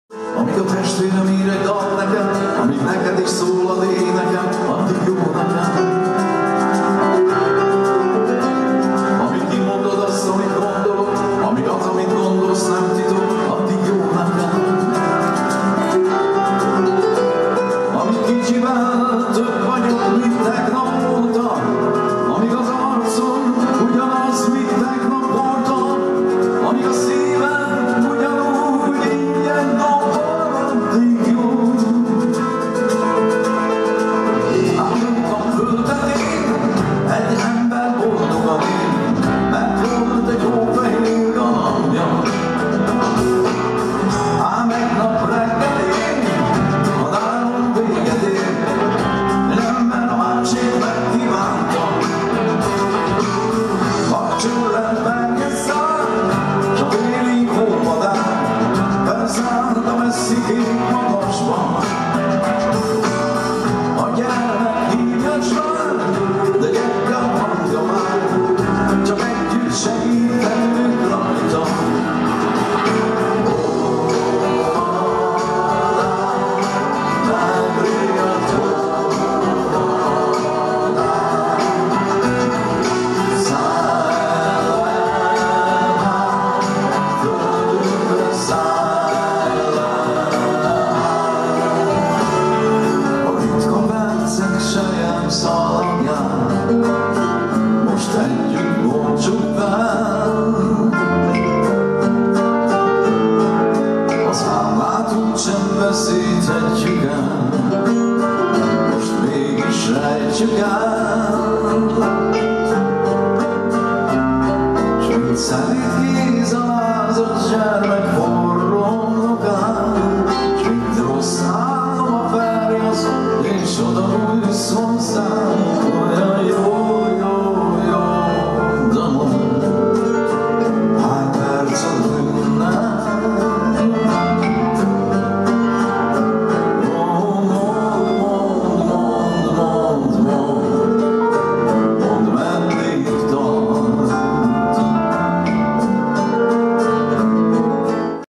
Zorán vypredal kultúrny dom v Dunajskej Strede
V Mestskom kultúrnom stredisku 2. apríla koncertoval Zorán. Vystúpenie jednej z určujúcich postáv maďarskej popovej hudby sa uskutočnilo ako náhrada za odročený koncert spred mesiaca pre zápal hlasiviek speváka. Zorán účinkoval v Dunajskej Strede v rámci turné pozostávajúce z 27 koncertov. Obecenstvo vypredaného kultúrneho domou tlieskalo 77-ročnému interpretovi a jeho 6-člennej sprievodnej skupine hudobníkov.